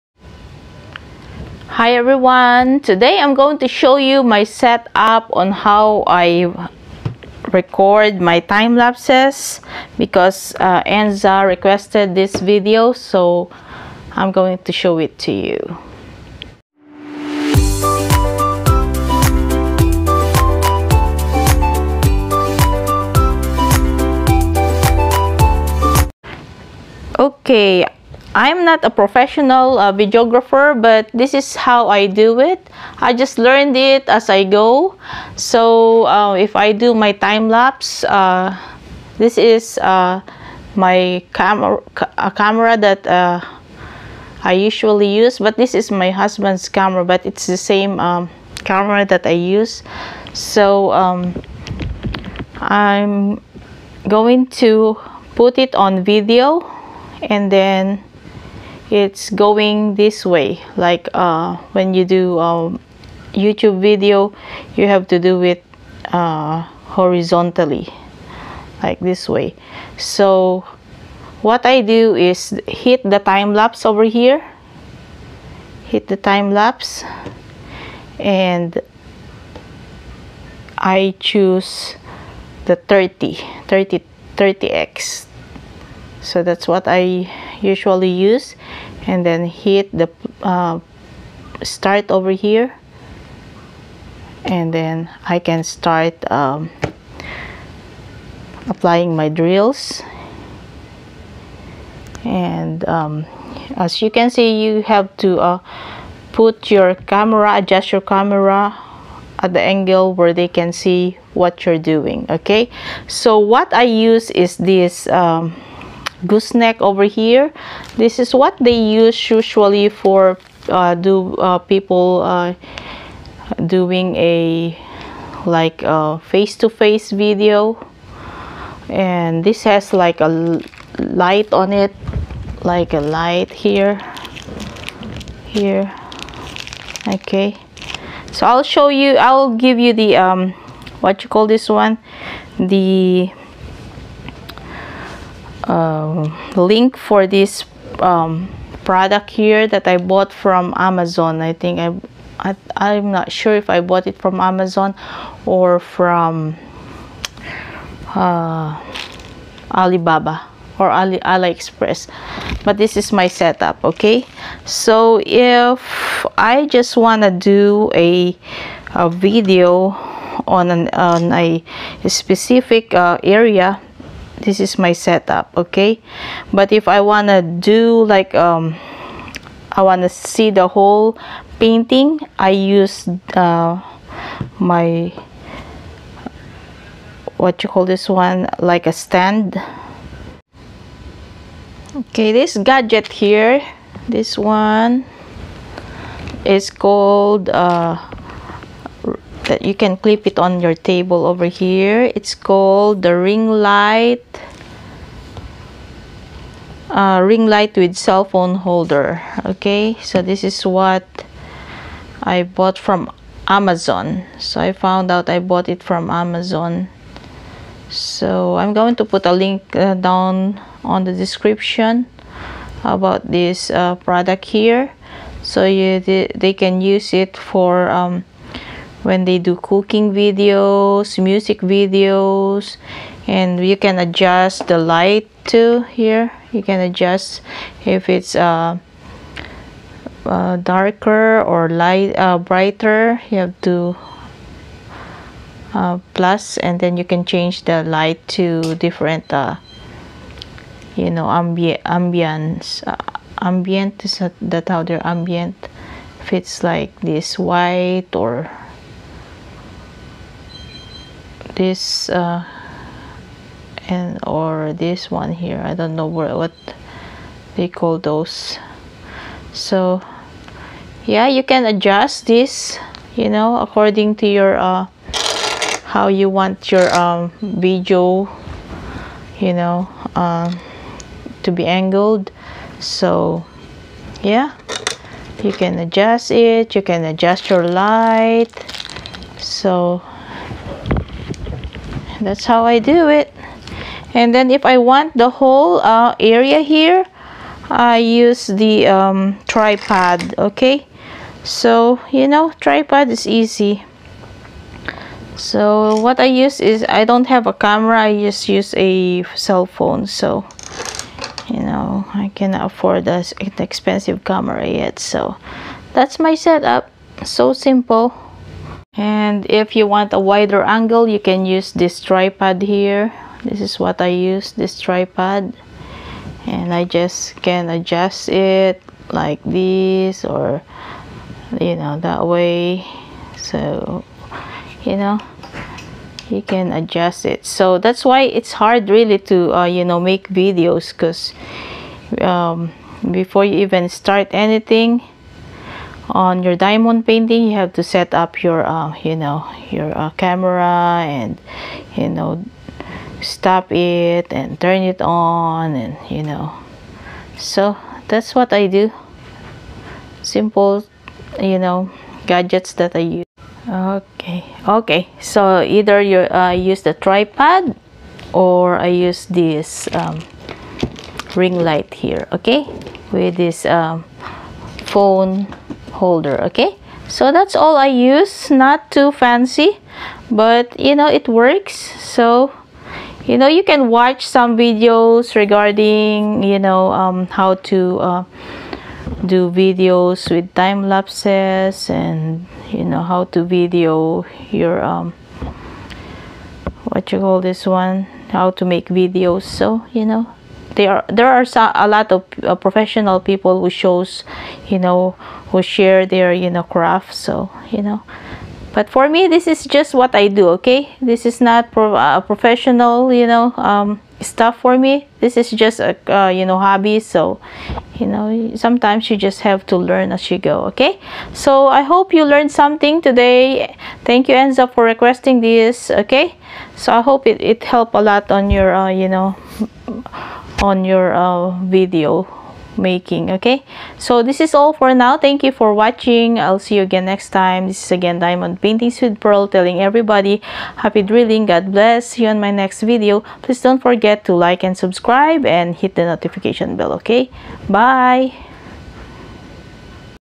How I do a time lapse video of my diamond paintings
Enza of @Enza's diamond painting Gallery requested for me to make a video on how l do my time lapse videos ... l am not a professional videographer but l hope you can learn something and you can also suggest on how l can improve my video recording. Thank you 💕

Ring Light with Cell Phone Holder...